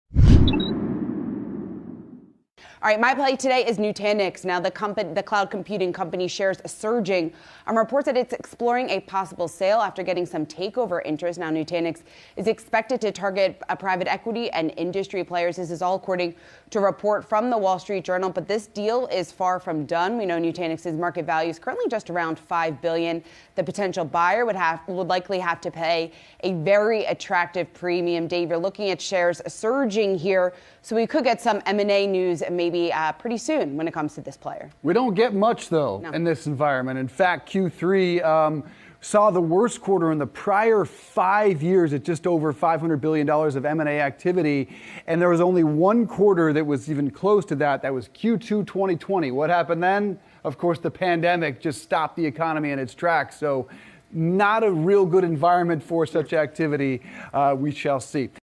Nutanix stock soars as company explores sale
Yahoo Finance Live anchors discuss the rise in stock for Nutanix amid reports that the company is exploring a sale.